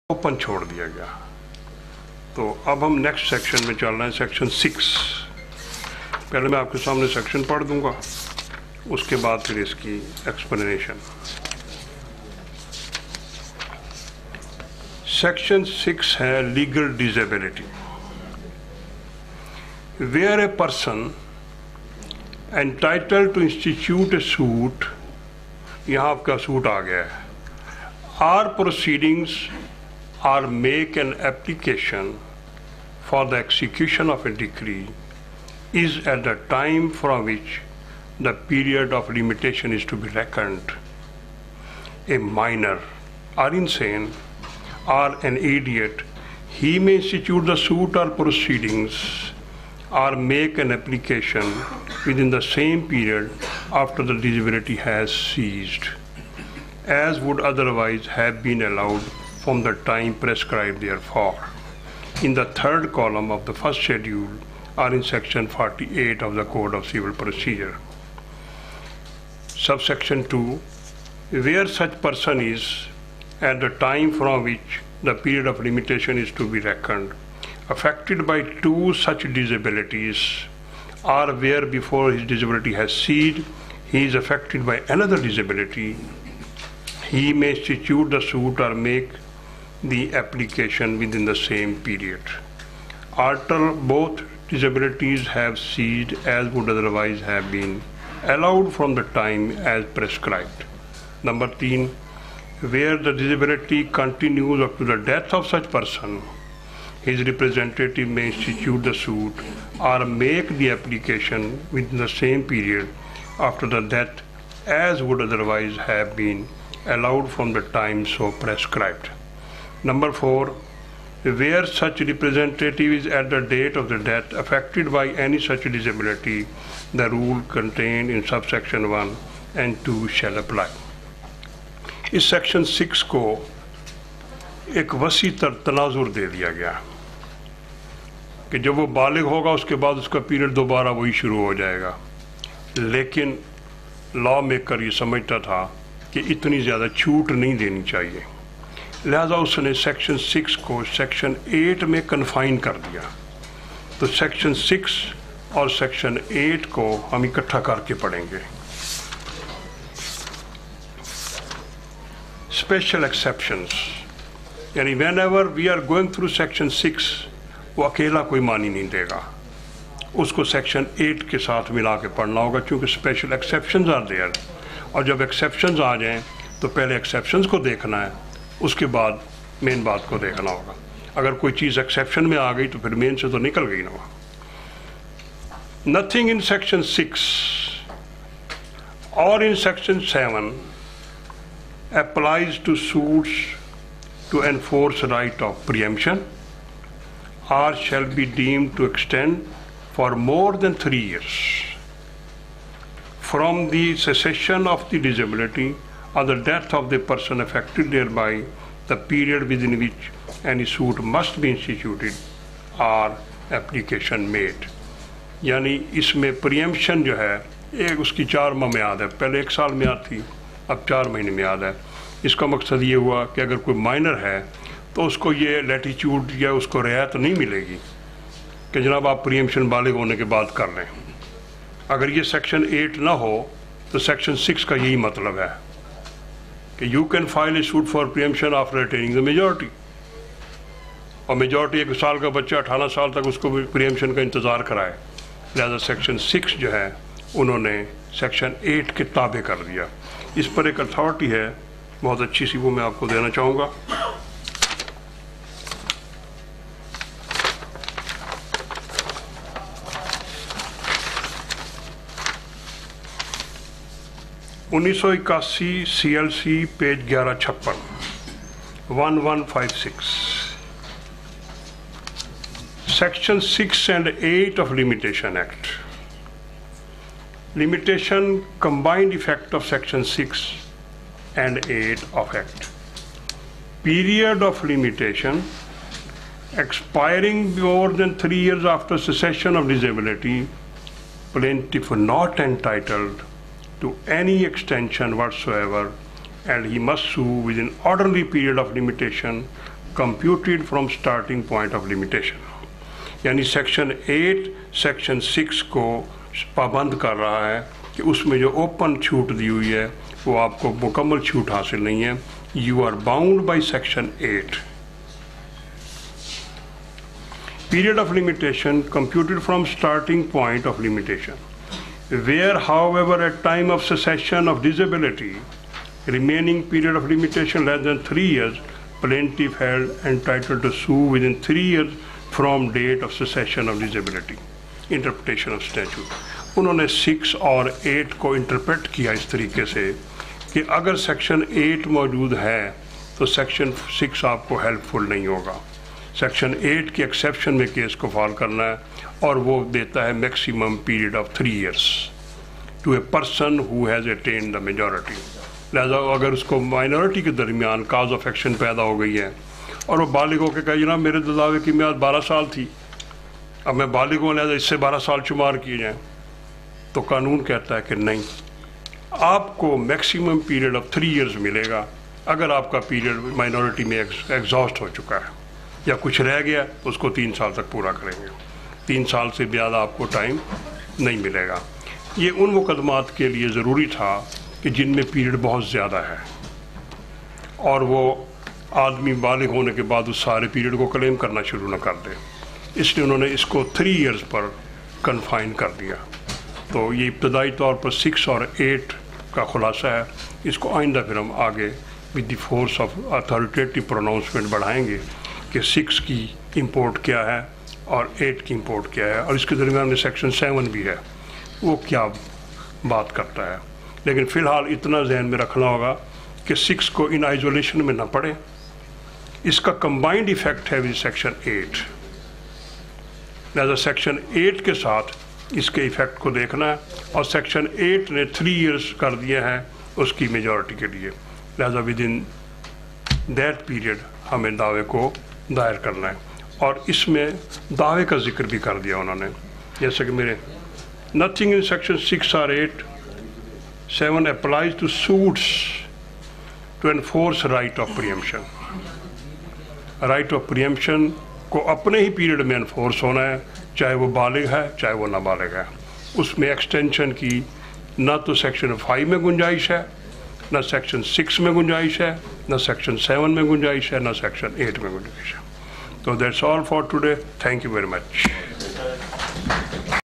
Section 6 with Section 8 of Limitation Act by Mutahir Tirimzi Advocate
Description:

In this video, Mutahir Tirimzi Advocate explains the interplay between Section 6 and Section 8 of the Limitation Act. Learn how these sections address situations involving minors, the insane, or individuals under legal disabilities and how these factors influence the computation of time limits for filing suits or applications. The discussion covers critical legal principles and their practical application in ensuring justice while considering unique circumstances. This lecture is perfect for law students, legal professionals, and anyone looking to deepen their understanding of limitation laws in Pakistan.